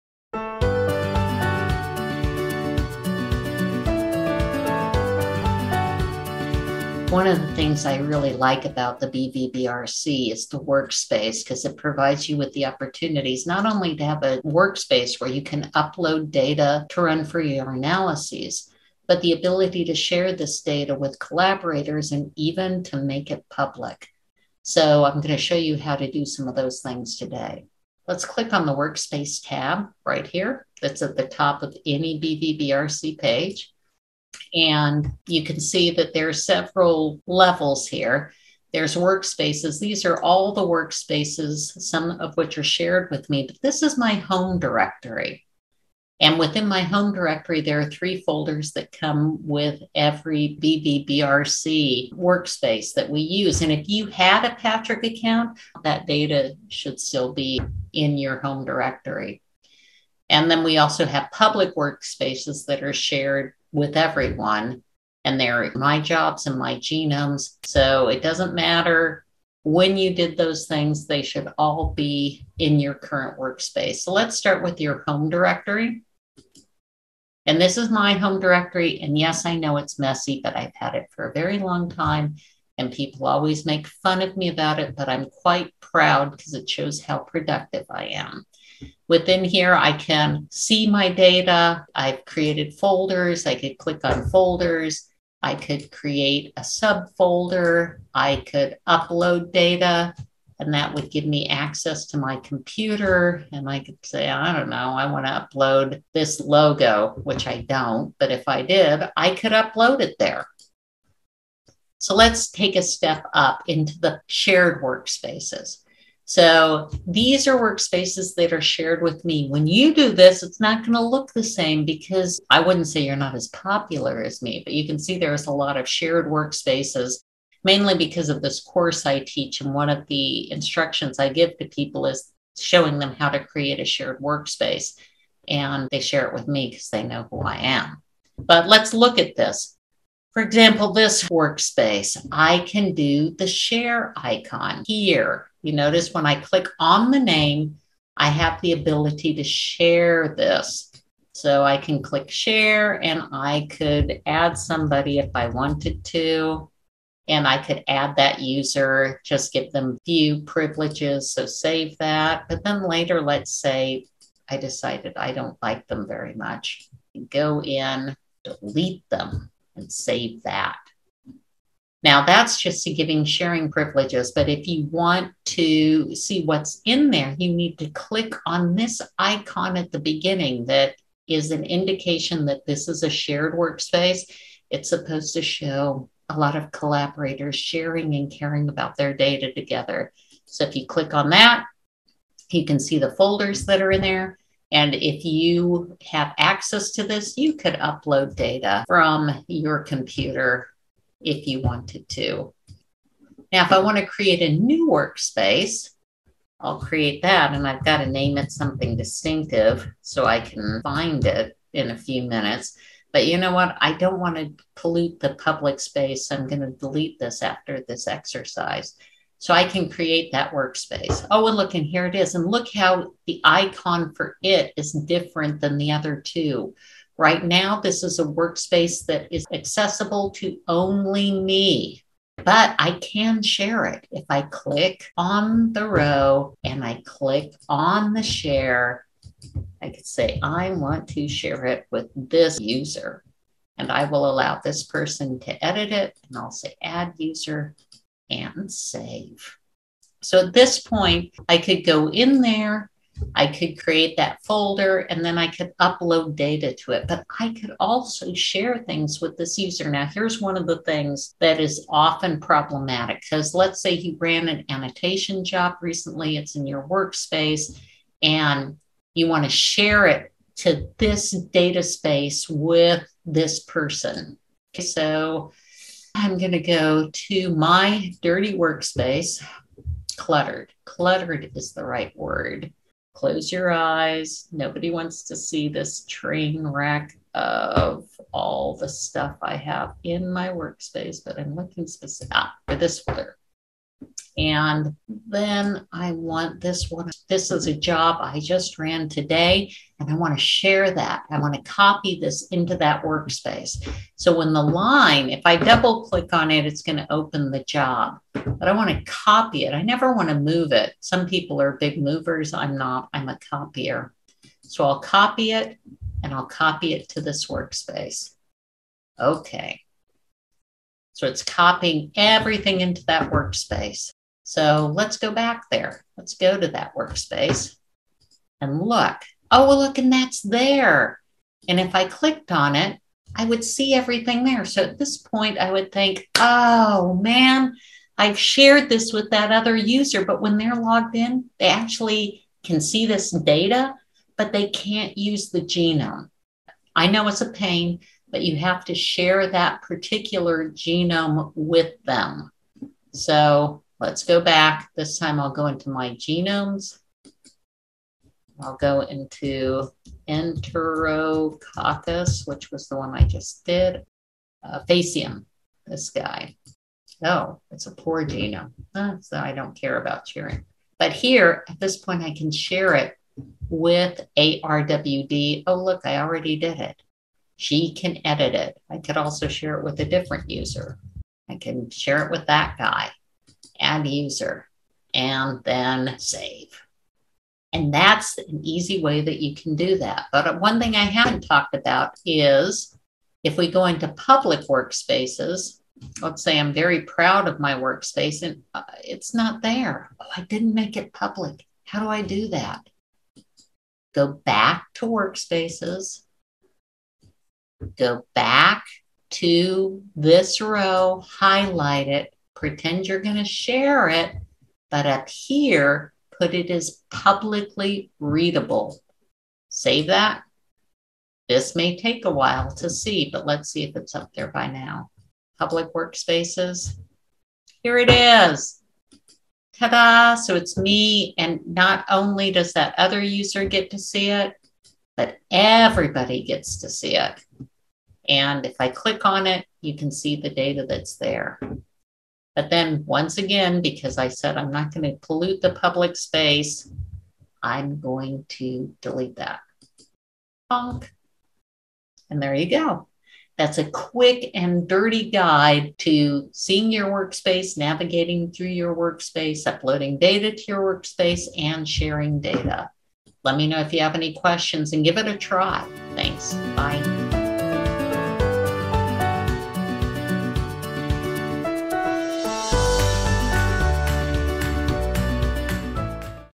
BV-BRC Workspace Guide
Overview of navigating the BV-BRC Private Workspace, sharing workspaces with others, and making workspaces publicly accessible.